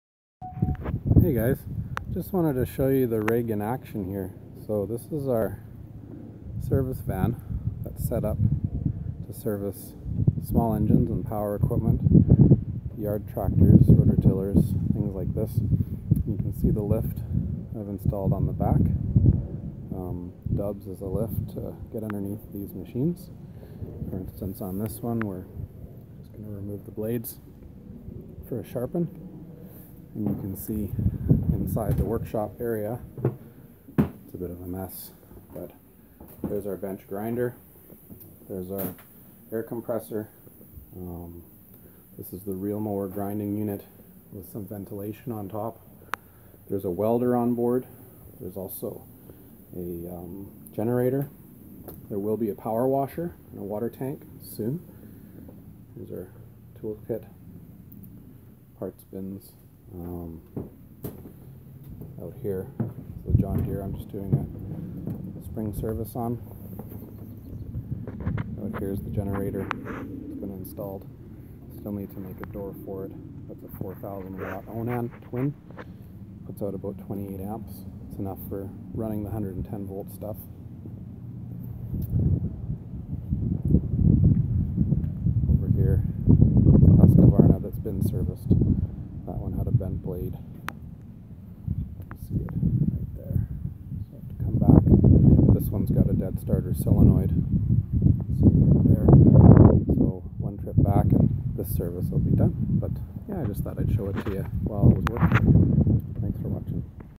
The Grumman Mobile lawnmower shop
Here is a quick look at how this project is coming along. Mobile Grumman lawnmower repair shop